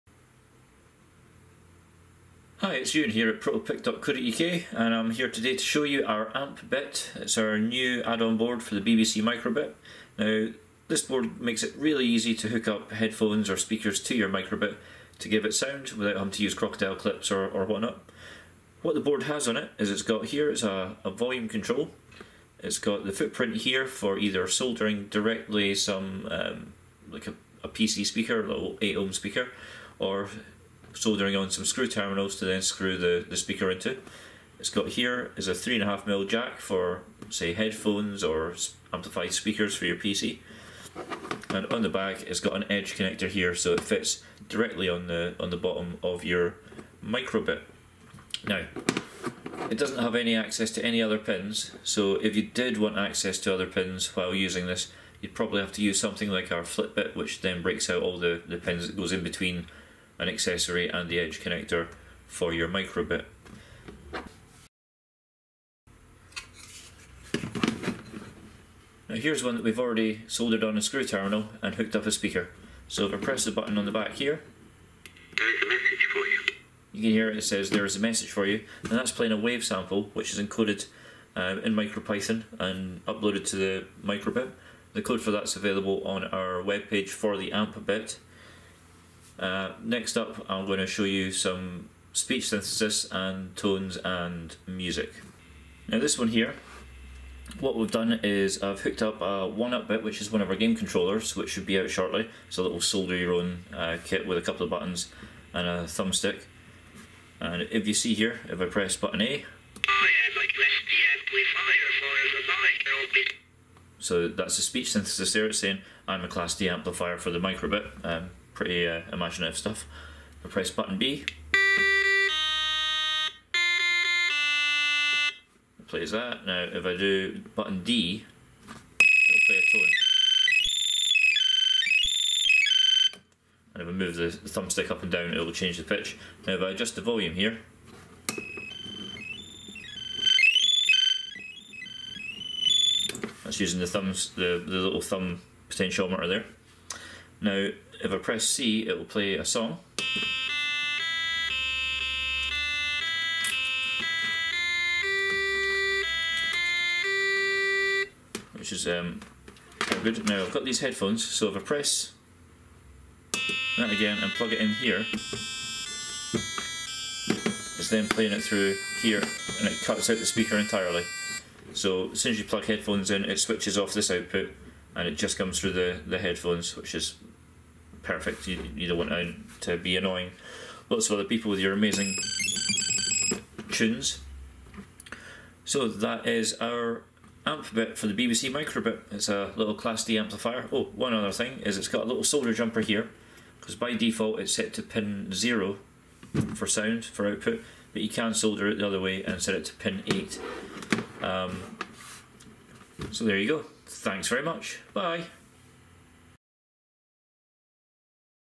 The amp:bit headphone and speaker add on for BBC micro:bit
This short video introduces the amp:bit, the easiest way to hook up a set of headphones or speakers to your BBC micro:bit. Simply plug the micro:bit into the amp:bit and then plug in your headphones!
alternatively you can solder a PC speaker directly to the provided pads or solder a screw terminal.
The amp:bit also features a built in volume control!

And if you need a speaker?! check these bad boys out